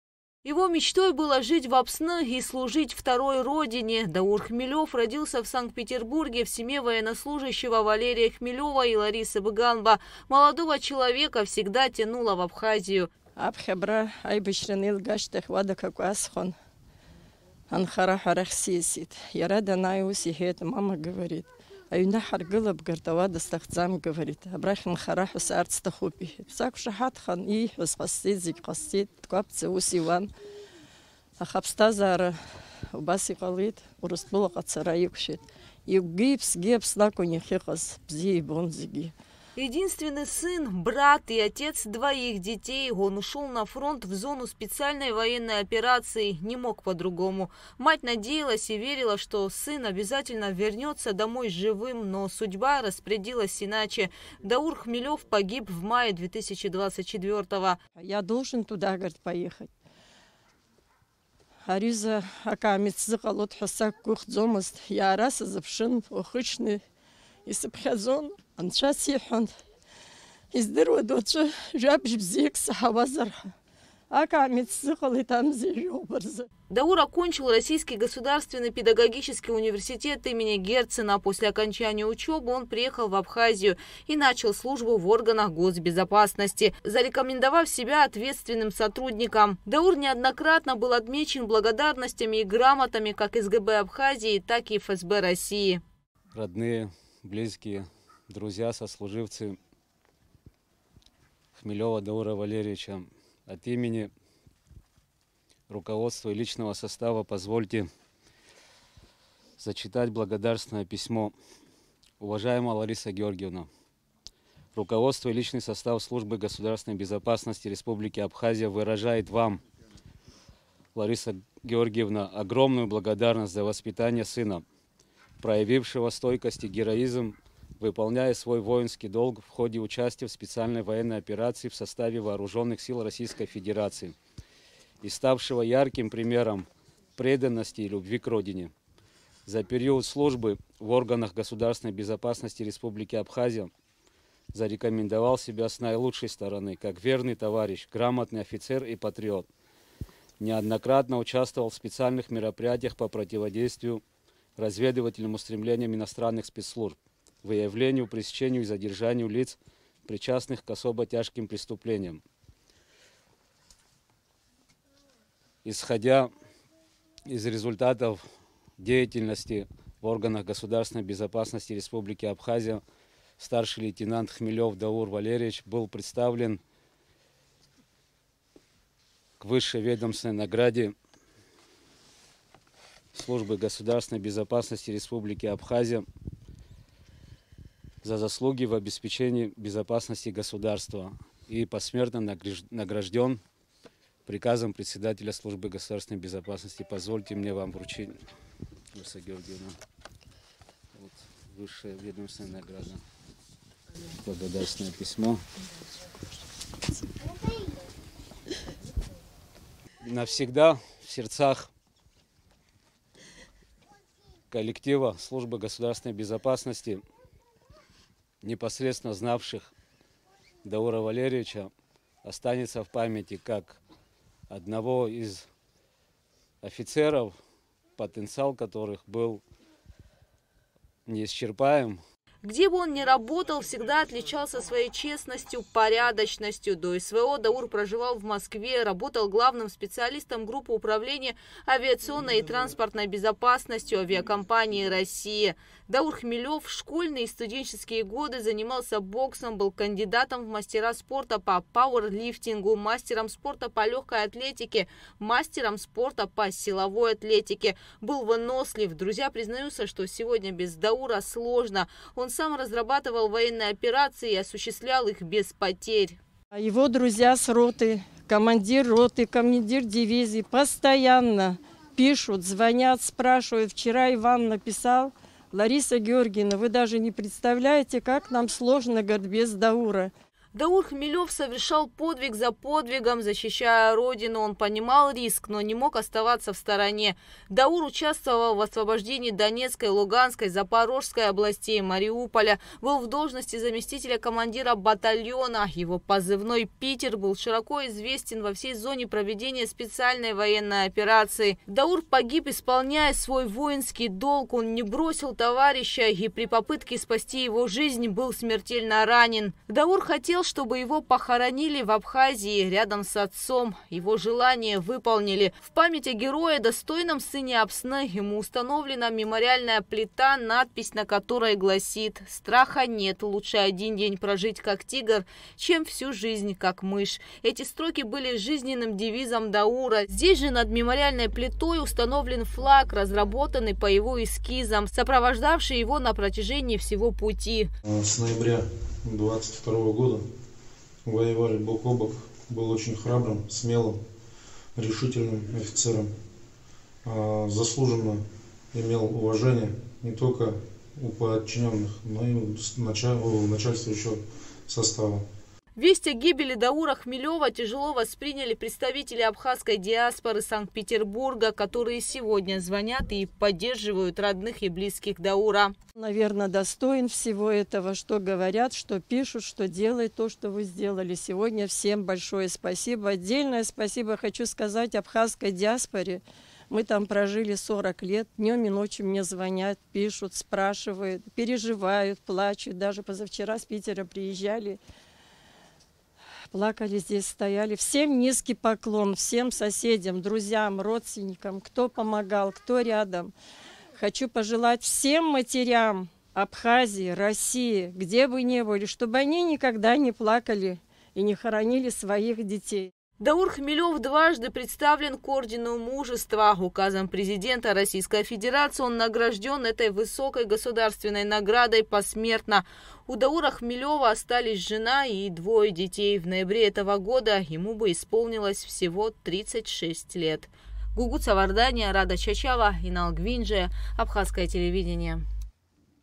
Памяти Даура Хмелева
Полгода назад, спасая жизнь товарища он был смертельно ранен в зоне специальной военной операции. Даур Хмелев был одним из тех, кто не мог оставаться в стороне, и считал своим долгом служить Родине, у него их было две Абхазия и Россия. Даур Хмелев был надёжным боевым товарищем, смелым, отважным, решительным. Наш следующий сюжет посвящён памяти героя.